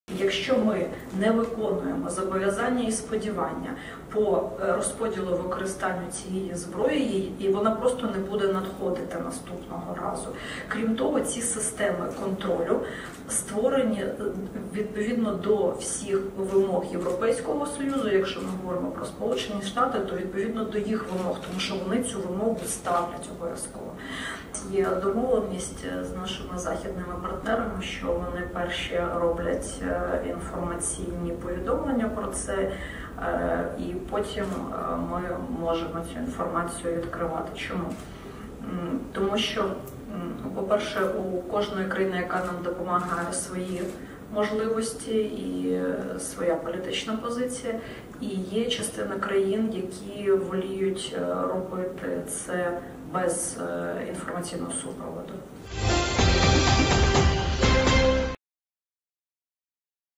Оружие под контролем
Запад ставит Украине жесткие требования по использованию предоставленного вооружения. Кроме того, именно Европа и США, исходя из собственных соображений, решают, когда  Киев может раскрывать информацию о получении военной помощи. Об этом на регулярном брифинге заявила заместитель министра обороны Украины Анна Маляр. В четверг ведомство объявило о получении четырех РСЗО HIMARS со стандартными снарядами дальностью полета до 80 км. Вашингтон поставил их Киеву с условием не наносить удары по российской территории.